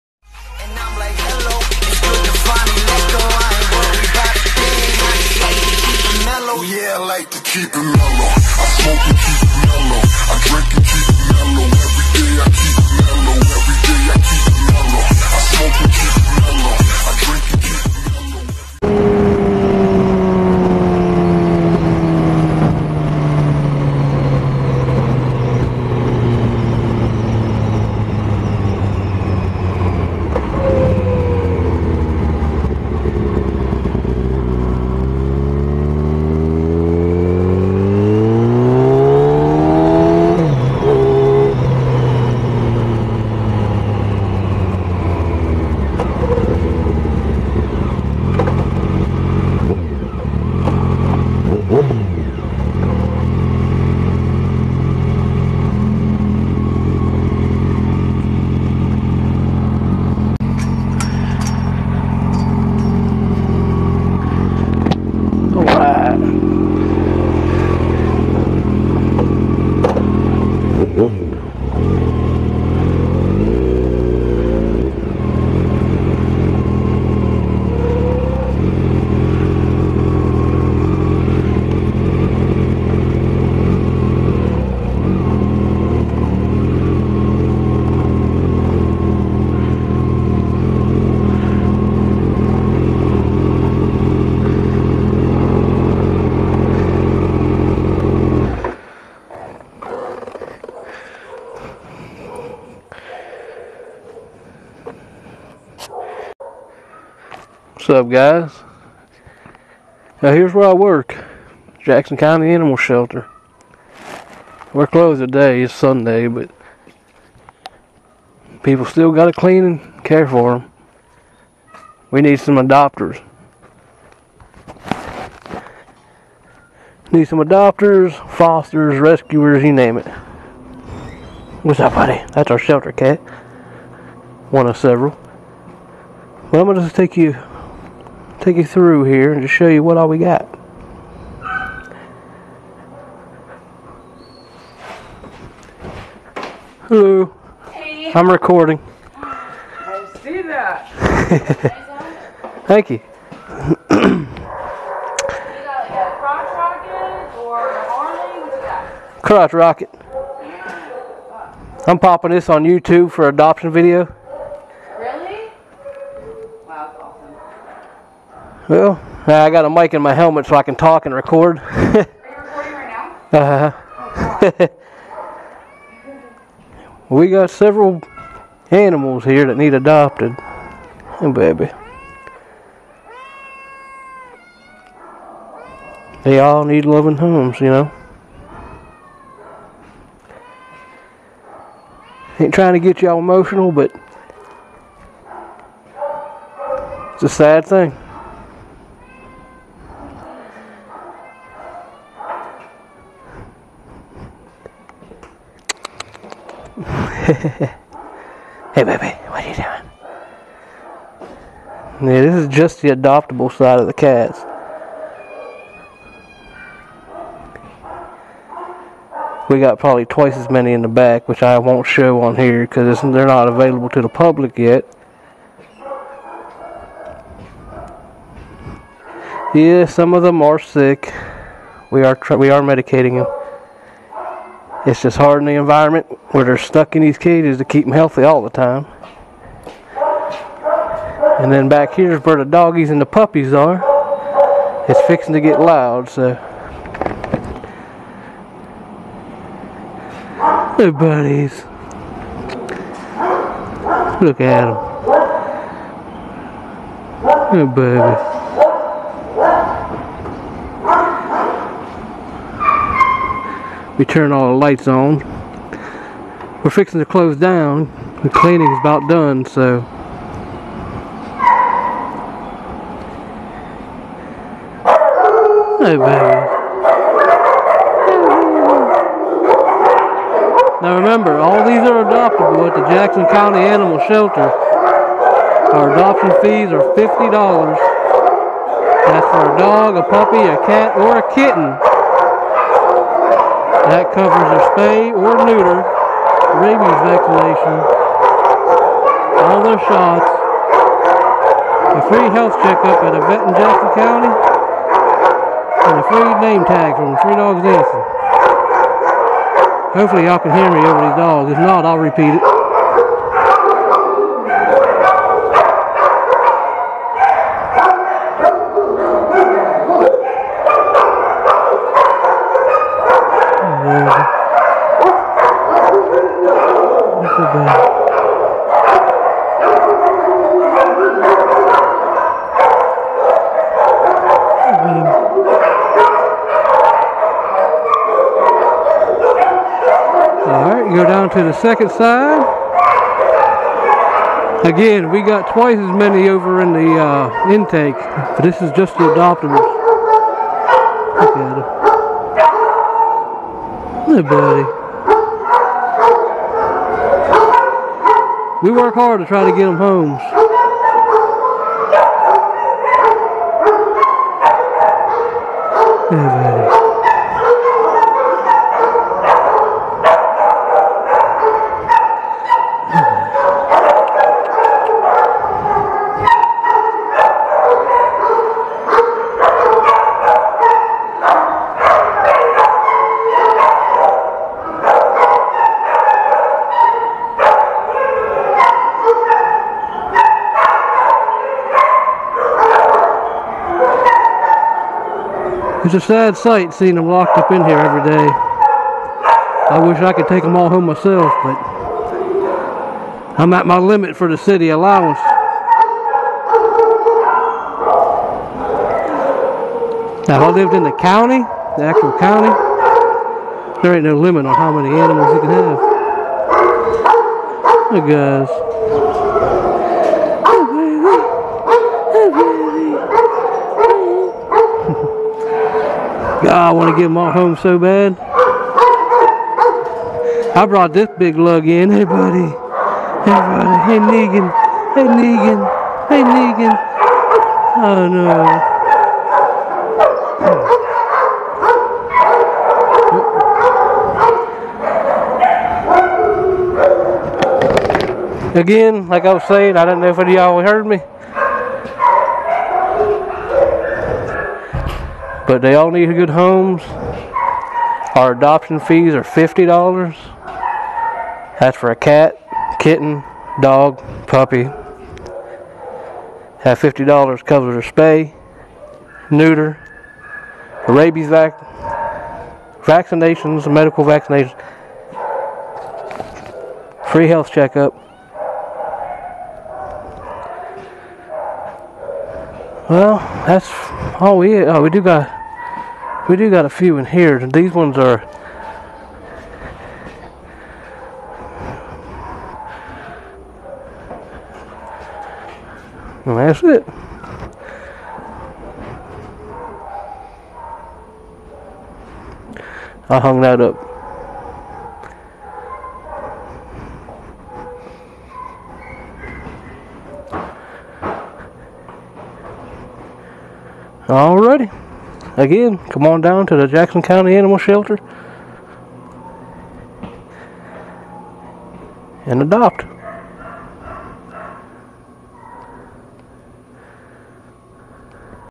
Today I visited the Animal Shelter.
Come down and give these babies a forever home.